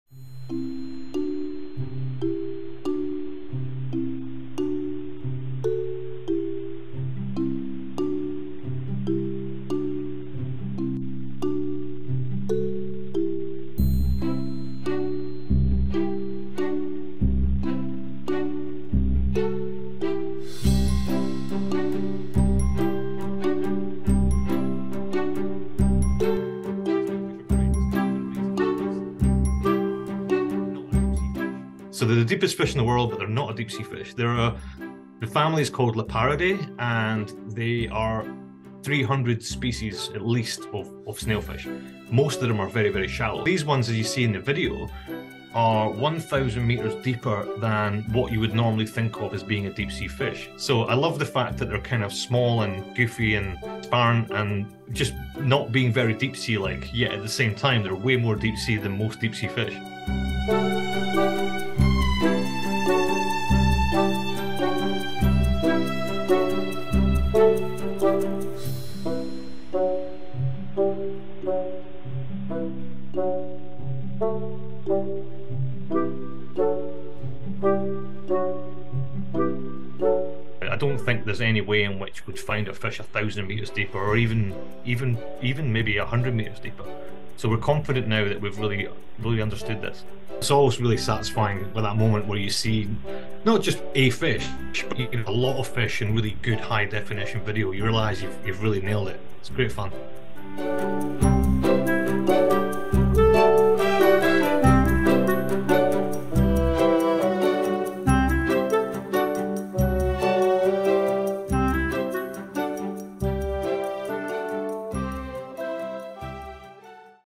Scientists break separate records for deepest fish ever caught and filmed near Japan
Scientists have broken the world records for the deepest fish ever caught, as well as catching such a feat on camera. As part of a joint Japanese-Australian expedition to study the deepest fish populations in the world, snailfish were filmed in August 2022 at a depth of 8,336 metres (27,349ft) in the Izu-Ogasawara Trench off the coast of Japan, beating the previous record in the Mariana Trench by 158 metres (518ft). Snailfish were also caught at a similar depth, the first time fish have been caught more than five miles (8km) below the ocean surface.